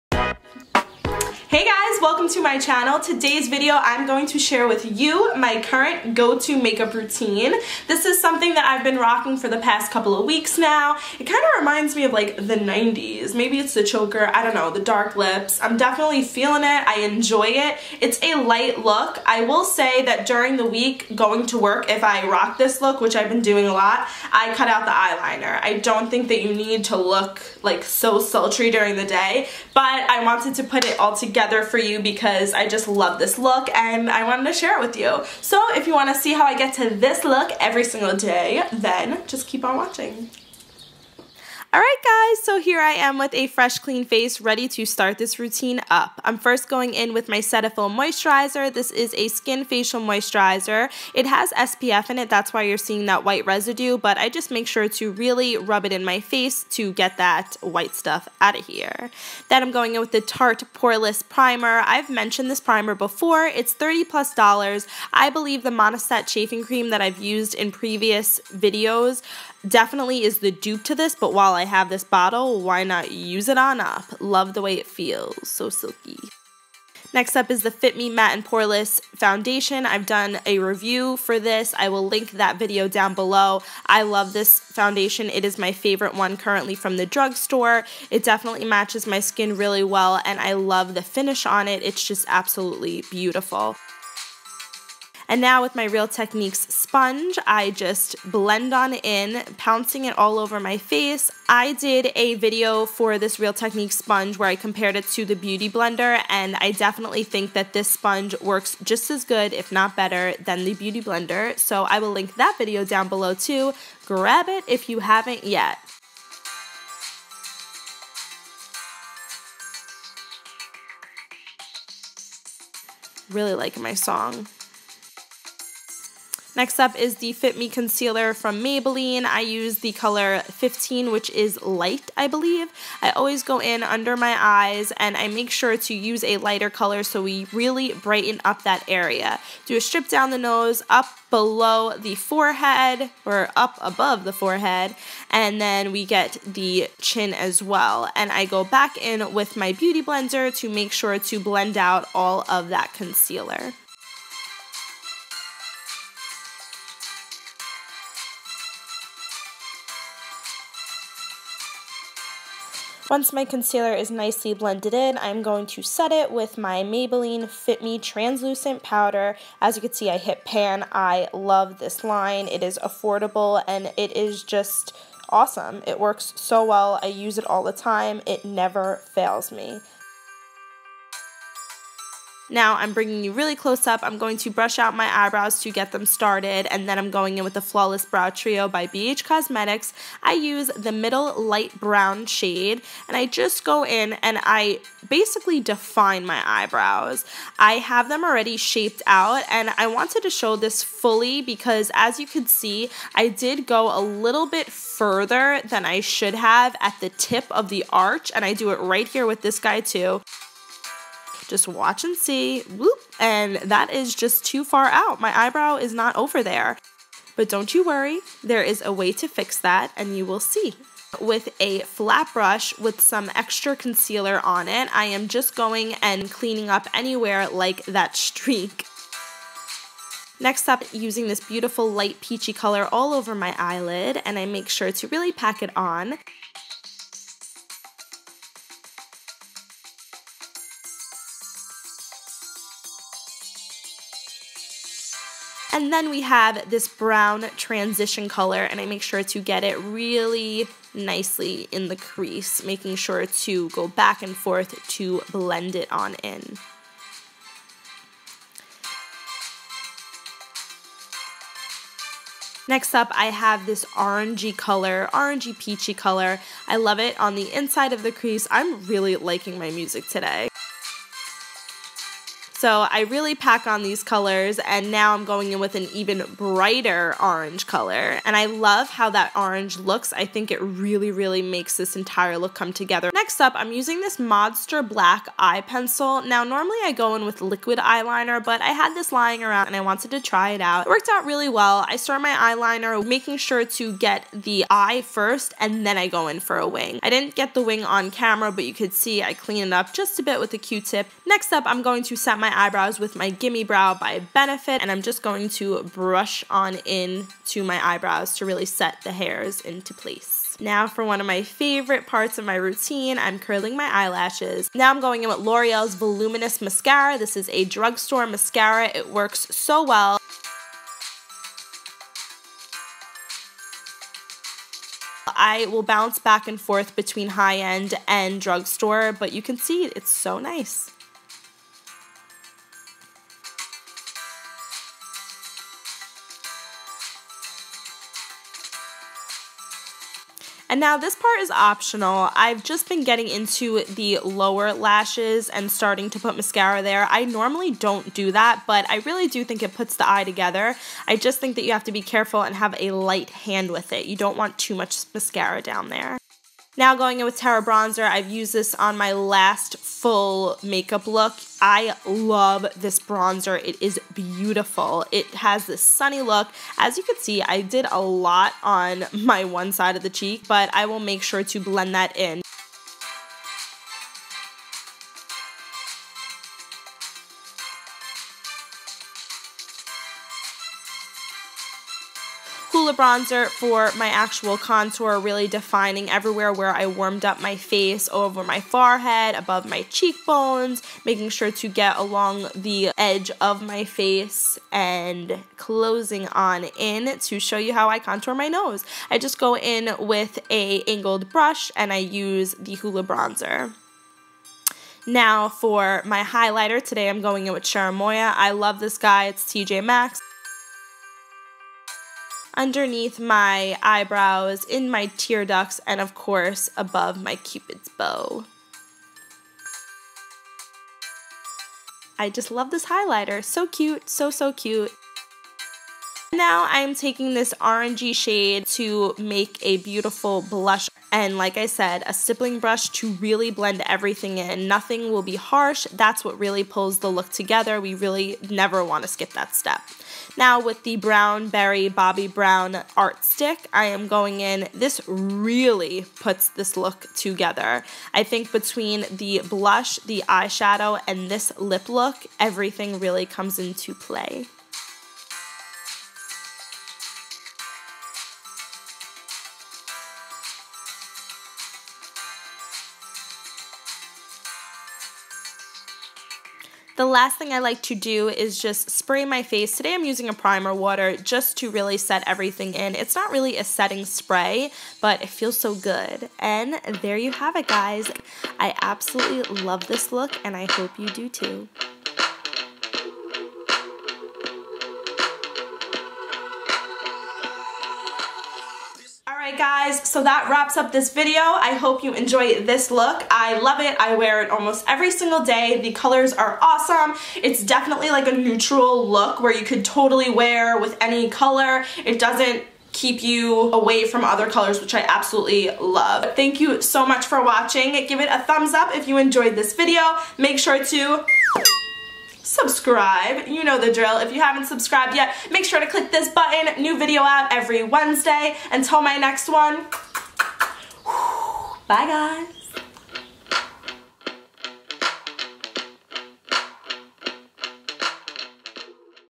MY CURRENT GO-TO LOOK -GET READY WITH ME!
Hey guys!!! I wanted to share with you my go-to look that I have been loving for the past few weeks. I hope you enjoy!

ALL PRODUCTS USED ARE LISTED DOWN BELOW (: !

Snap: LB1788

Products Used:

F A C E -  P R E P

Moisturizer : Cetaphil Facial Moisturizer
Primer: Tarte Clean Slate Primer

Foundation: Maybelline Fit Me Matte & Poreless Foundation - Natural Beige
Concealer: Maybelline Fit Me – Light
Setting Powder: Maybelline Fit Me Setting Powder -Translucent
Bronzer: Terra Bronzer
Contour: Benefit Hoola
Blush: BH Cosmetics “Professional Blush Palette” – Coral/peach color
Highlighter: Cherimoya Contour Kit - Highlighter & Illuminator

E Y E S/ B R O W S

Mascara - L'Oreal Voluminous
Eye Liner - Modster eye pencil in black
Brows: Flawless Brow Trio BH Cosmetics
Eyelids: BH Cosmetics “Professional Blush Palette”

Lip - Bobbi Brown Art Stick - Berry Brown

S E T T I N G  S P R A Y
SmashBox Photo Finish

The End.